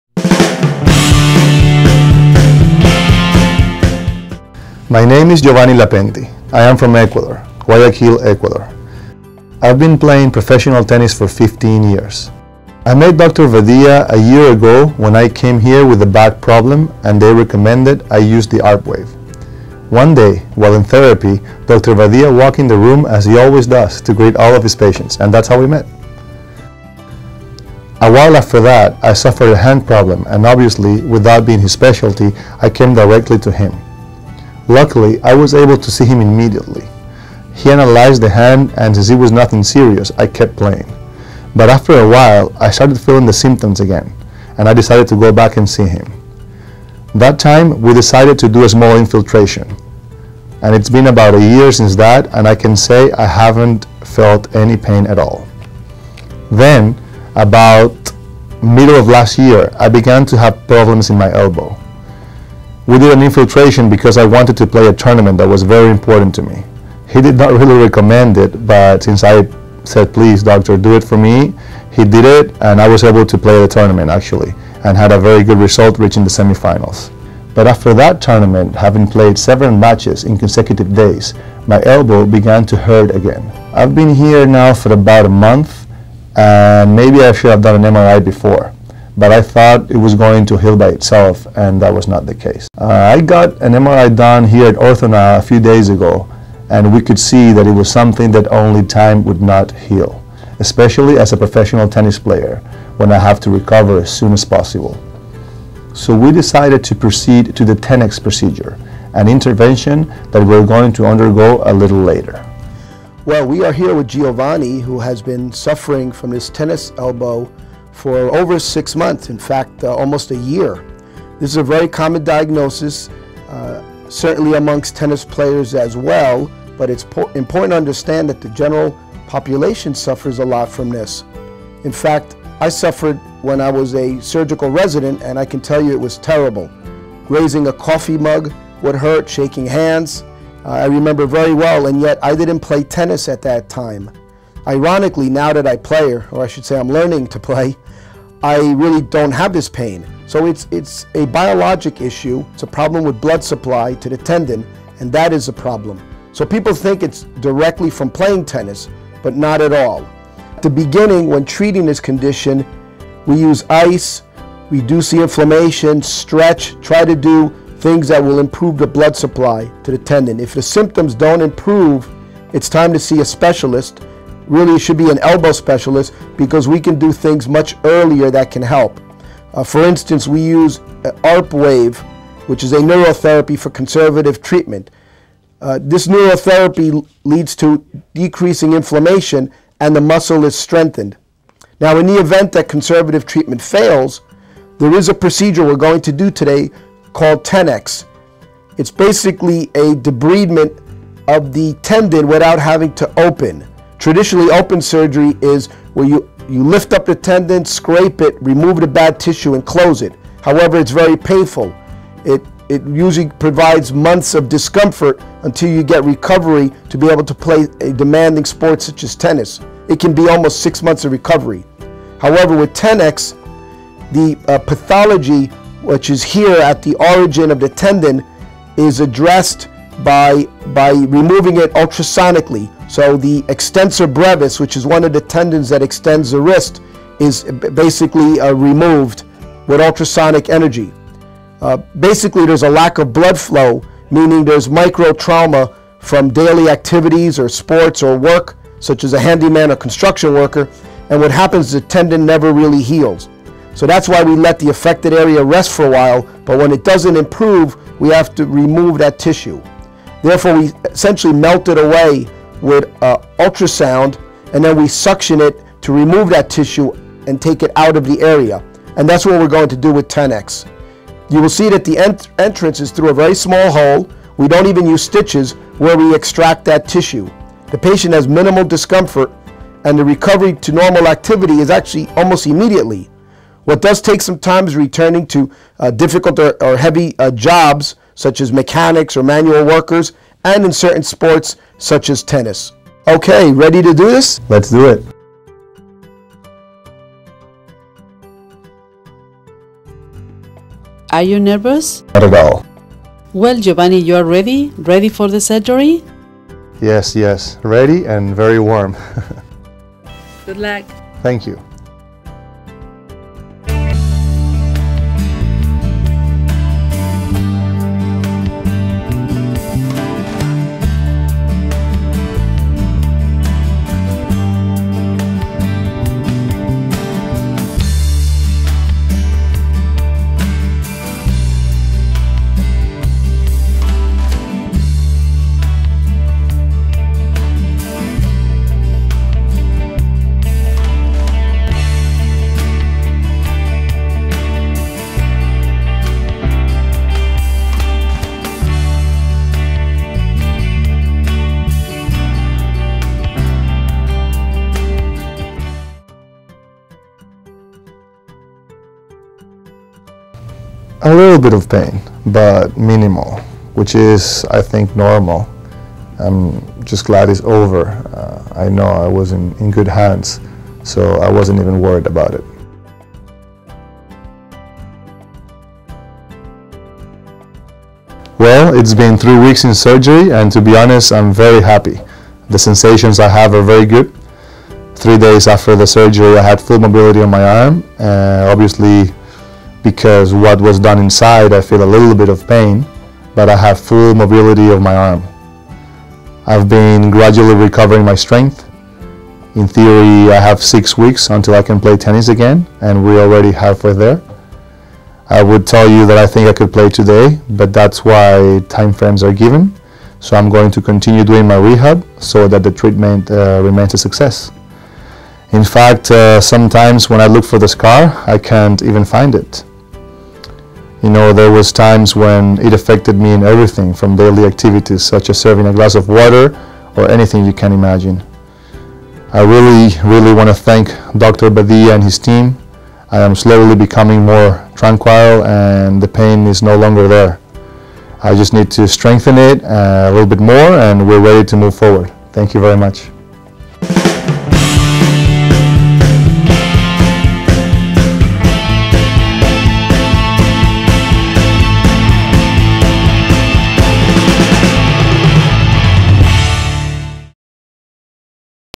Giovanni Lapentti - Professional Tennis player - Tennis elbow
This video shows Giovanni Lapentti before, during and after his treatment for tennis elbow problem

Giovanni Lapentti is a professional tennis player from Guayaquil, Ecuador. He had tennis elbow problem and Dr. Alejandro Badia recommended him the TENEX Procedure since he is an athlete and needs a minimally invasive intervention and fast recovery to solve his tennis elbow problem.

Watch the video for more details on initial procedure and follow-up.

3650 NW 82nd Ave. Suite 103
Doral, Florida 33166